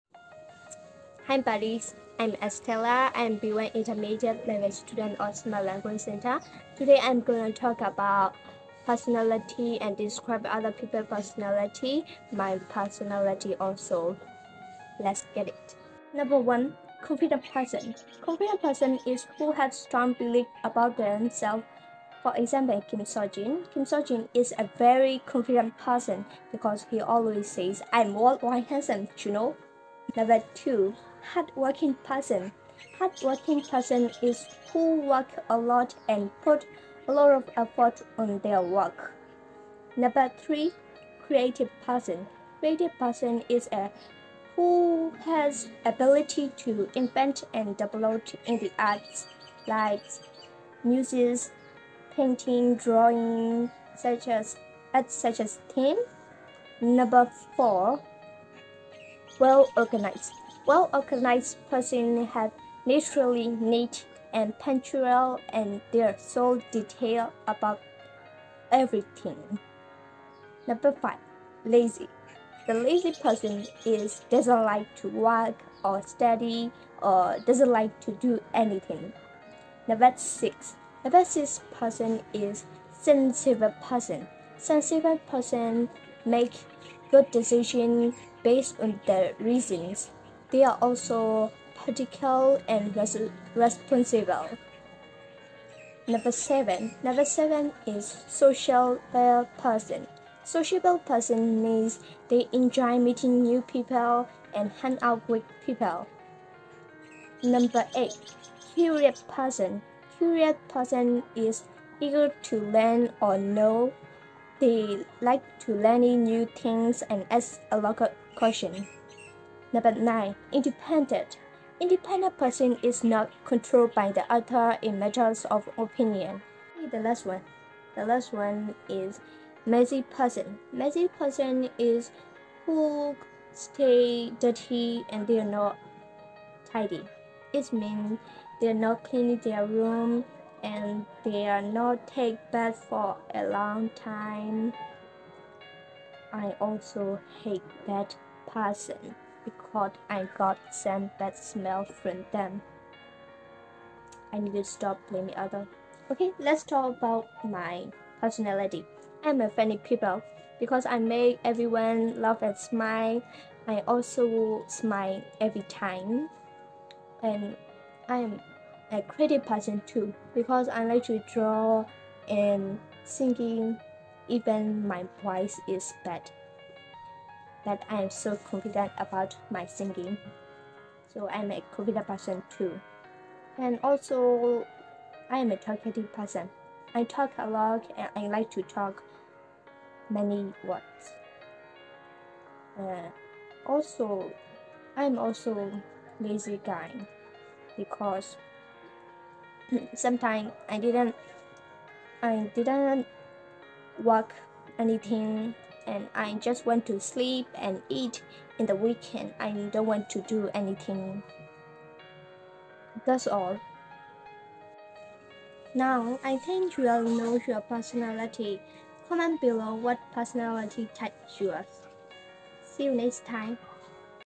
Student’s video project
B1 Intermediate Level Student, Estella is presenting on the topic of “Personality, Describing Her Personality and Others Personality”

After watching this video, you will gain the knowledge of what are personalities and know the type of yours personality 😉

Find it out🥳

If you like this video, you click the like button and follow our page too🥳😍